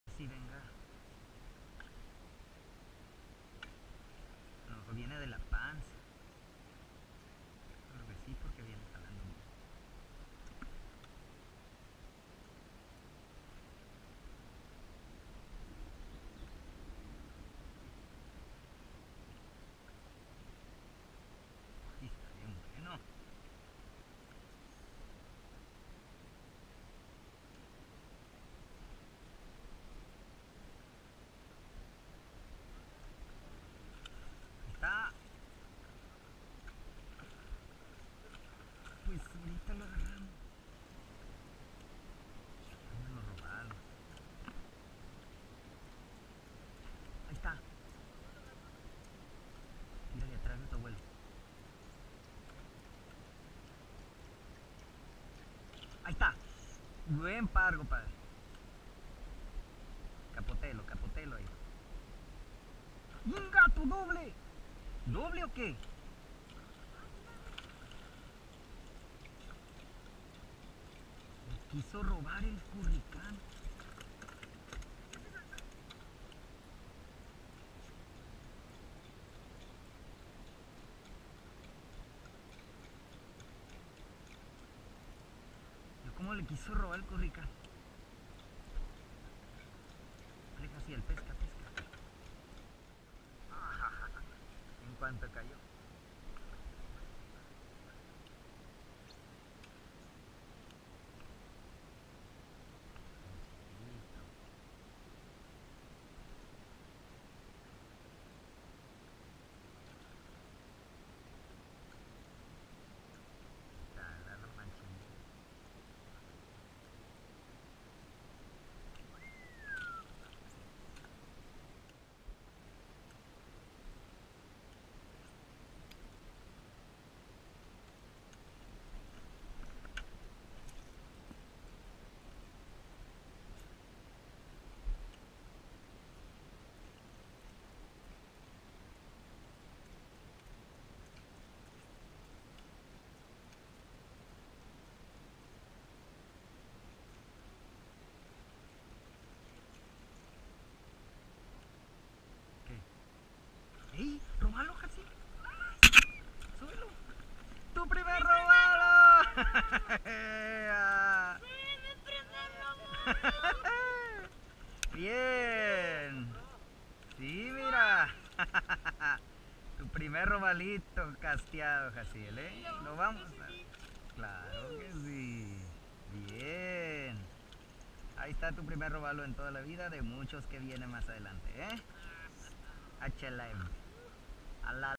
Primer robalo de mi hijo Jaziel// his first snook on mangloves
saca su primer robalito en esteros y varios pargos buenos en la pasada pesca...inmejorable compañero de pesca